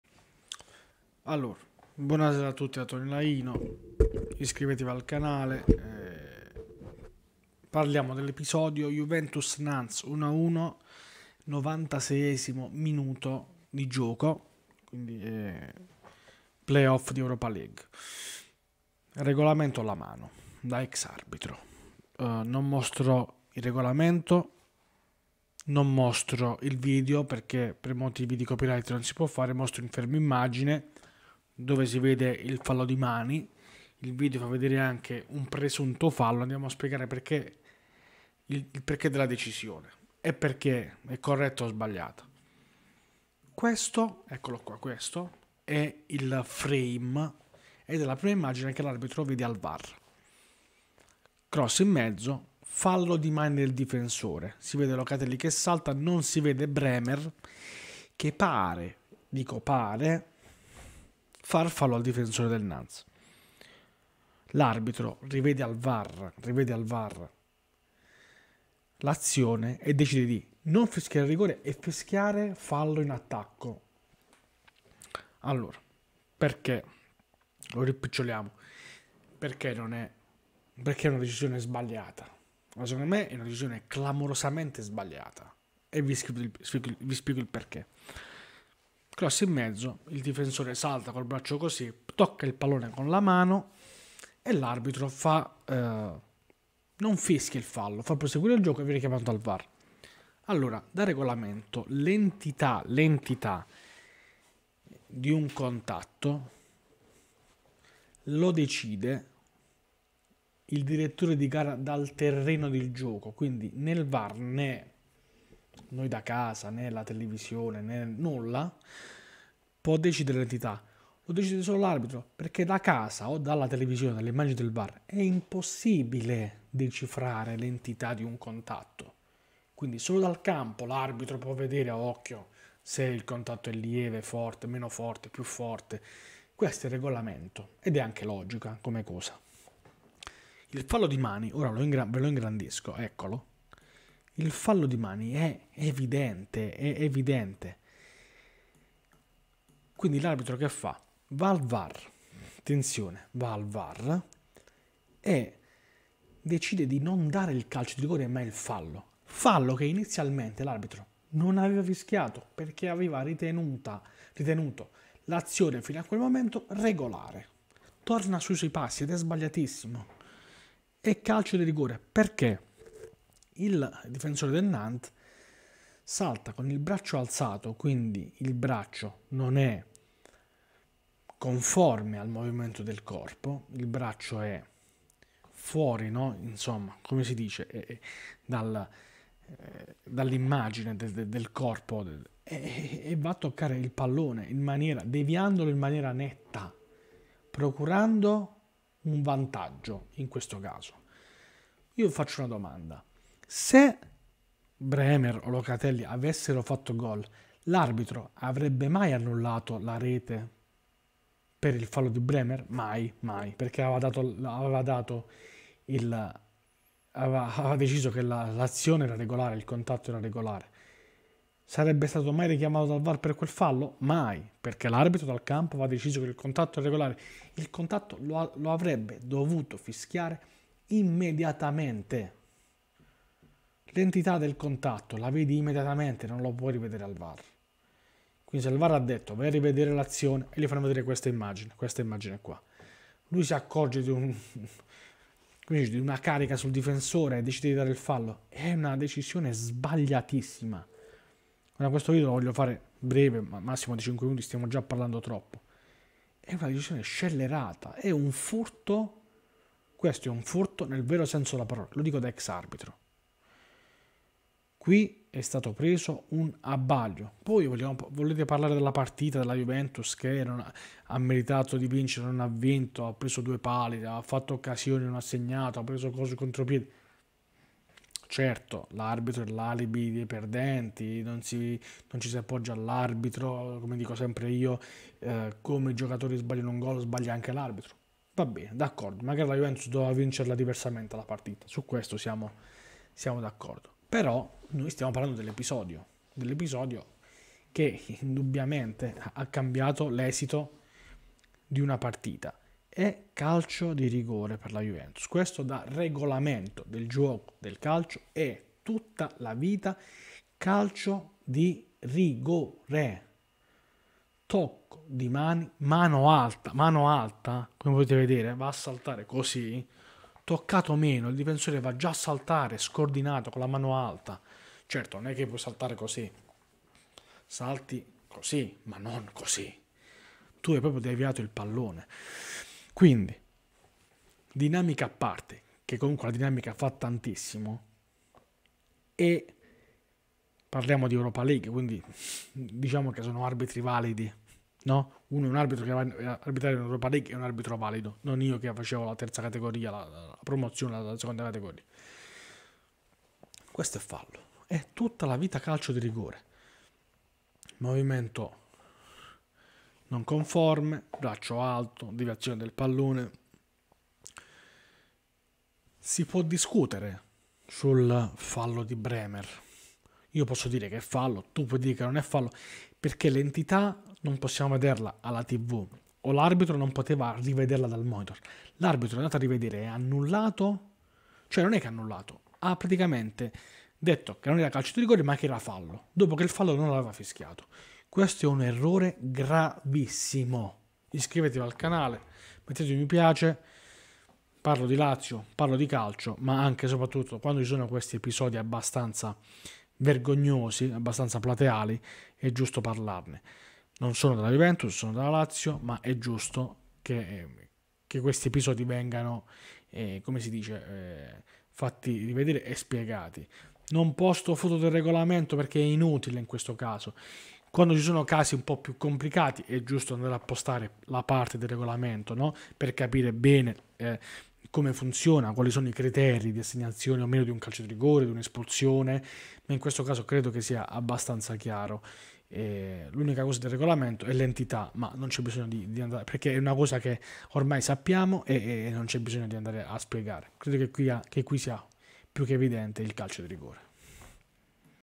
JUVENTUS-NANTES 1-1 | Analizziamo il RIGORE - L'episodio da MOVIOLA visto REGOLAMENTO ALLA MANO.
TUTTO SUL CAMPIONATO DI CALCIO